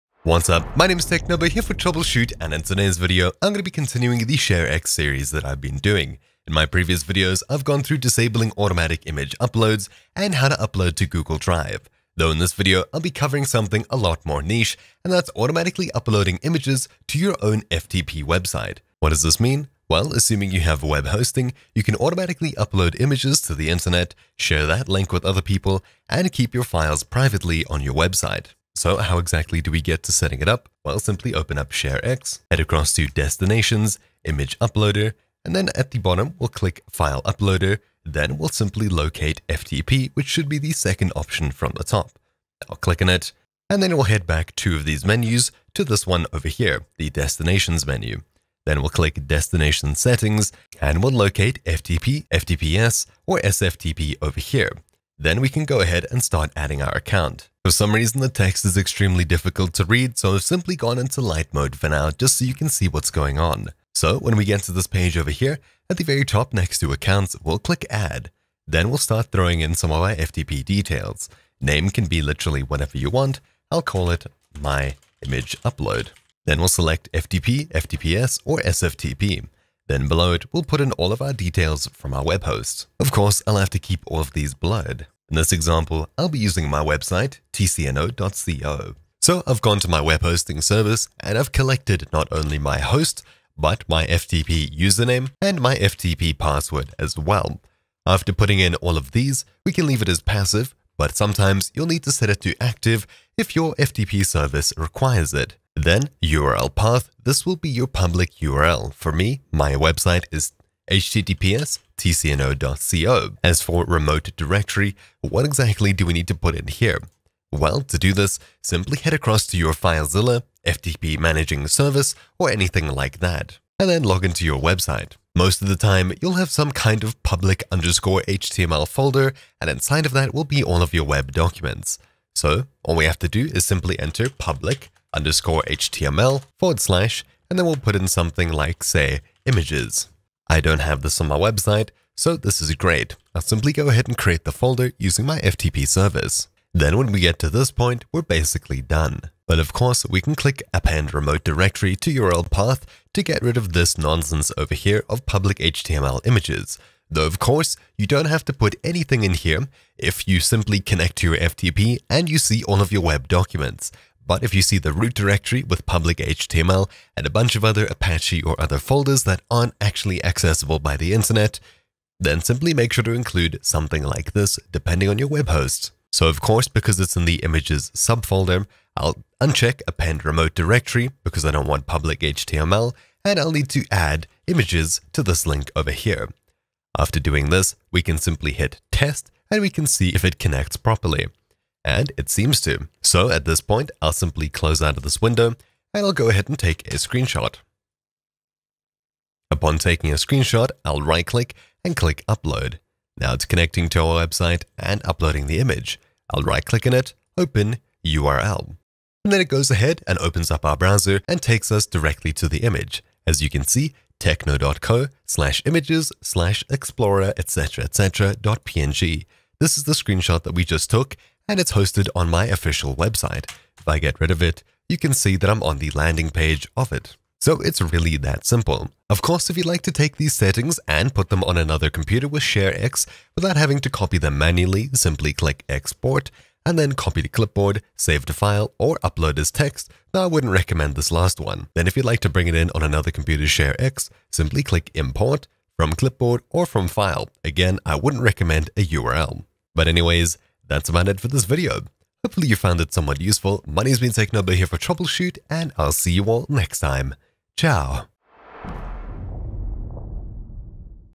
ShareX: Custom domain uploads | FTP guide for ShareX
This video should get you on your feet with automatic image uploads to your own domain using FTP and ShareX. Take a screenshot, and it'll appear on your website almost immediately, or of course, choose when to upload them to your website with a few clicks.

Timestamps:
0:00 - Explanation
0:28 - Setting up FTP Uploads in ShareX
1:03 - Adding an FTP Account
3:15 - Testing custom FTP upload
3:48 - Copying FTP upload settings to another computer